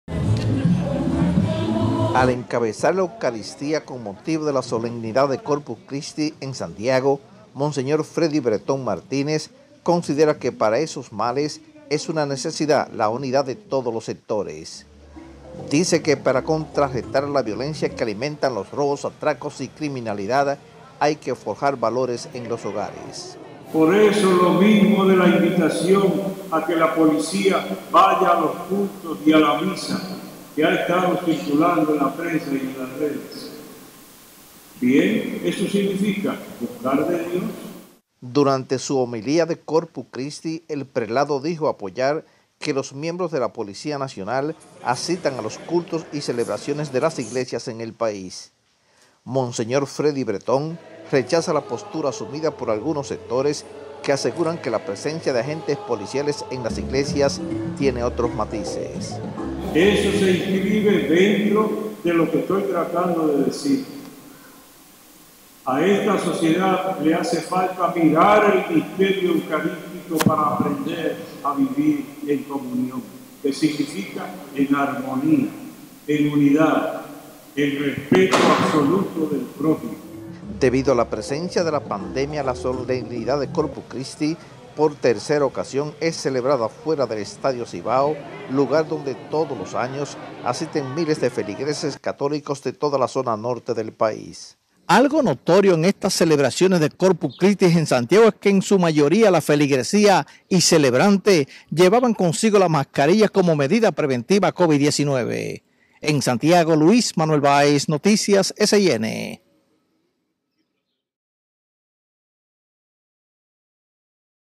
Eucaristía por Corpus Christi en Santiago
Arzobispo de la Arquidiócesis de Santiago, considera que los dominicanos deben de unirse para hacerle frente a los robos, violencia y criminalidad.
Al encabezar la eucaristía  con motivo de la solemnidad de  Corpus Christi en Santiago, monseñor Freddy Breton Martínez, considera que para esos males, es una  necesidad la unidad de todos los sectores.